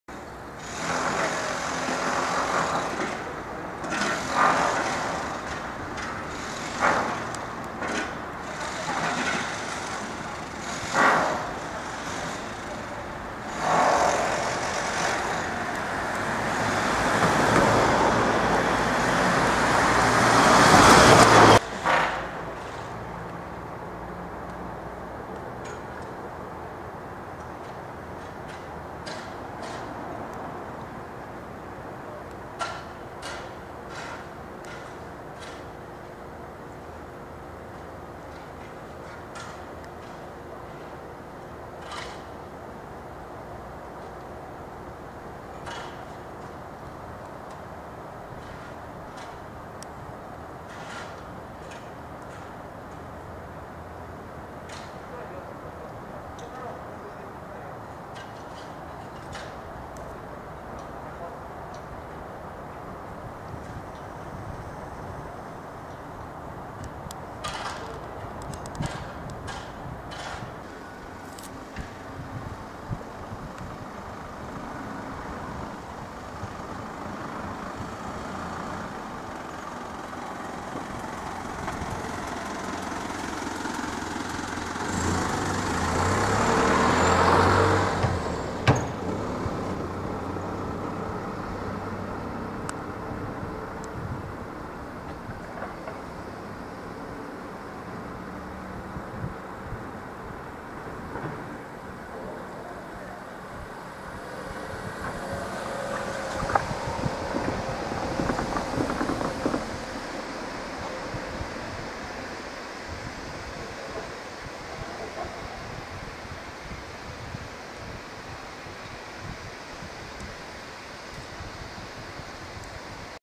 2013年5月川重兵庫工場詣で １
ＧＷ１０連休の工場は　ひっそり
していました。